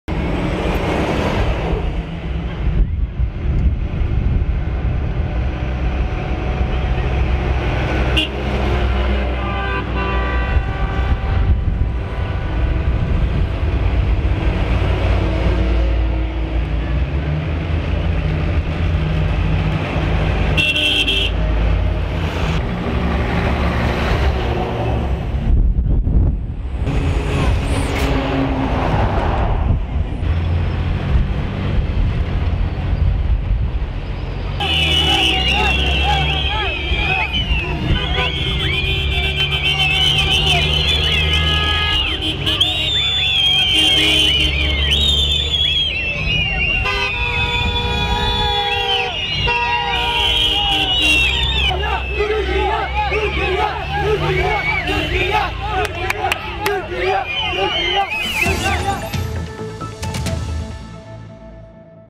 Suriye sınırına yaklaşık 130 araçlık konvoy
Yaklaşık 130 araçlık konvoyun Şanlıurfa'nın Akçakale ilçesine girişi sırasında vatandaşlar sevgi gösterisinde bulundu.

Konvoyun ilçeye girişi sırasında bazı vatandaşlar arabaların kornalarını çalarak, bazıları da el sallayarak sevgi gösterisinde bulundu.

Konvoyun Suriye sınırında konuşlu askeri birliklere takviye amacıyla gönderildiği öğrenildi.